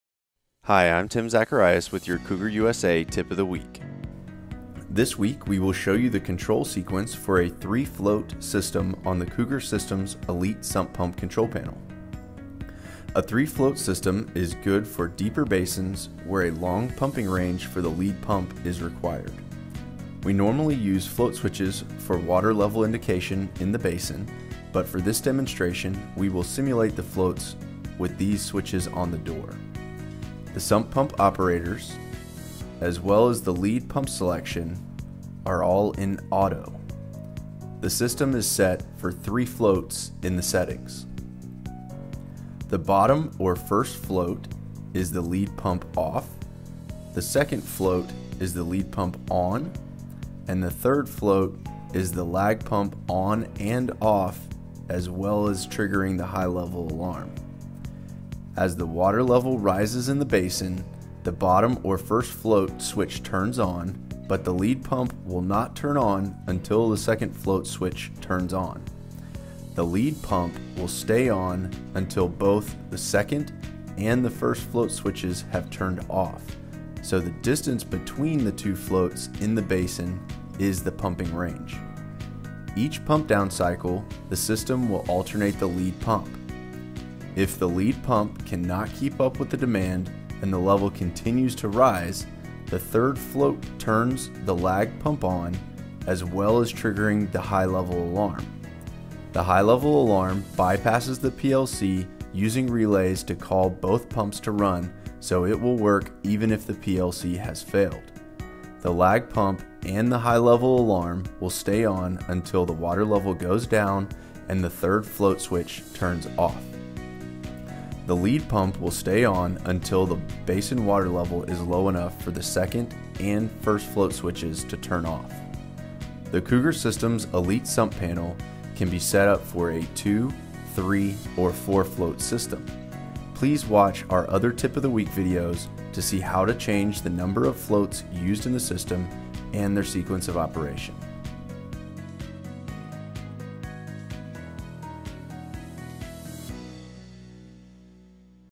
Elite Sump Pump Control Panel 3 Float Sequence - Cougar USA Tip of the Week
This week we will show you the control sequence for a 3 float system on the Cougar Systems Elite Sump Pump Control Panel.